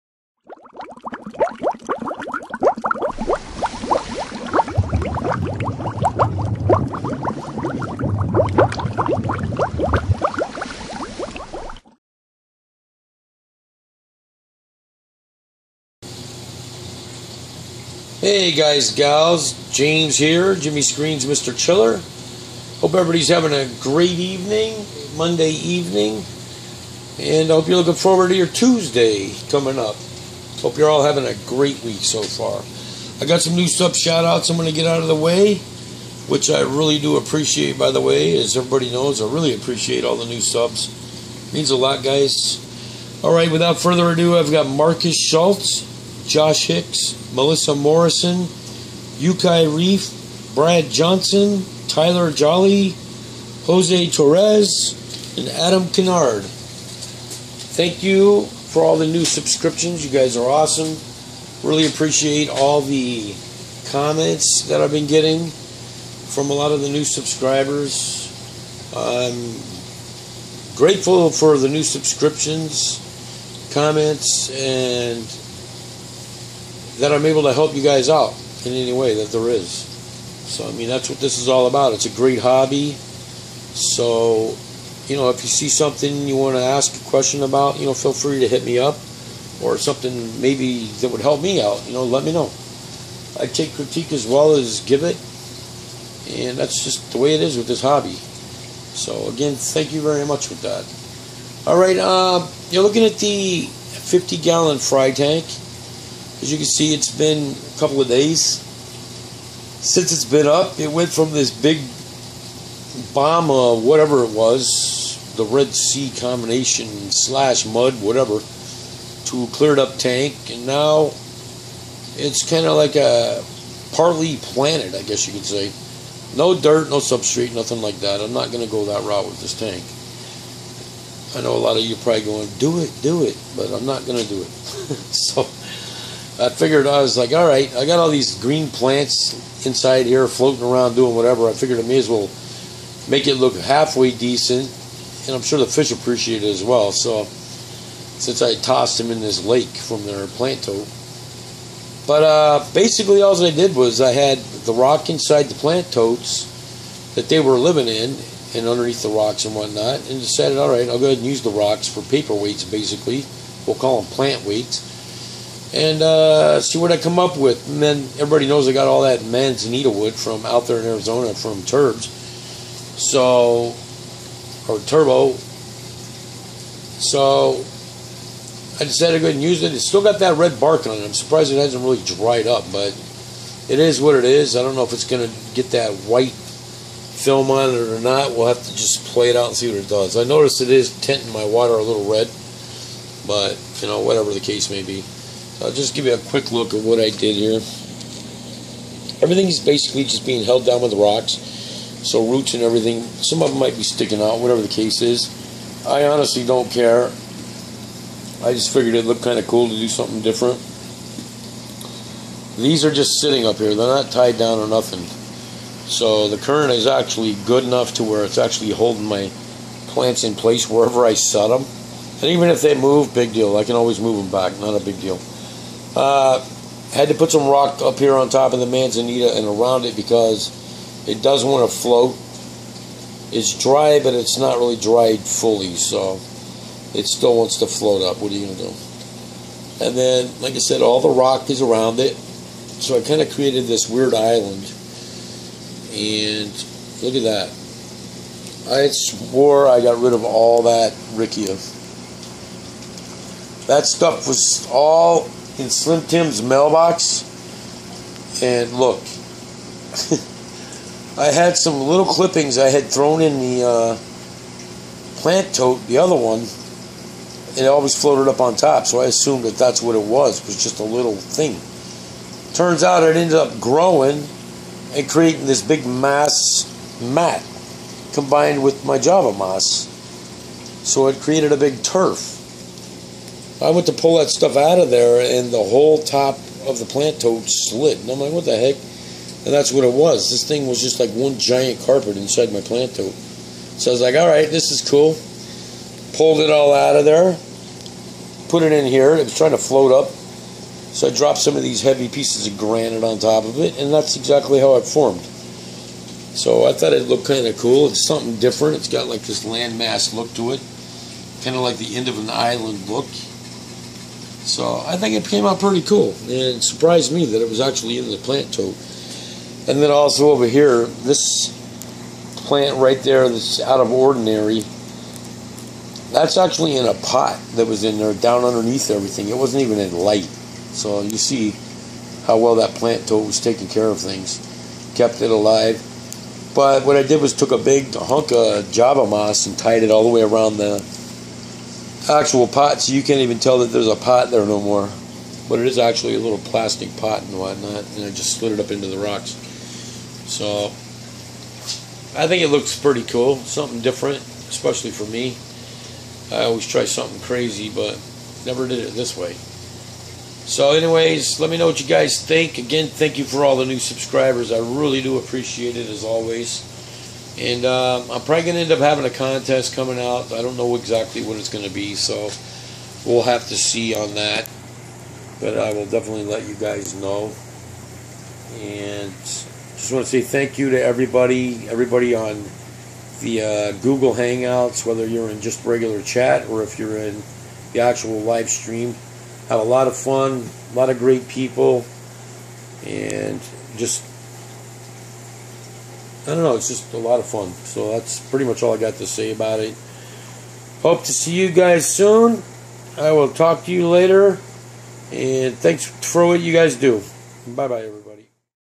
New Sub Shoutouts !! What I've been Up Too Pt.4
Hey guys n gals, thanks to all my new subscribers, as always greatly appreciated. Thanks for bringing my channel over the 700 mark, that's awesome. So thanks to everyone, I'm in the process of creating a contest for all my subs, which will probably consist of freshwater and saltwater, so stay tuned for that in the coming weeks.
Check Me out on Instagram
ManhattanReefs.com
FiredUpAquatics.com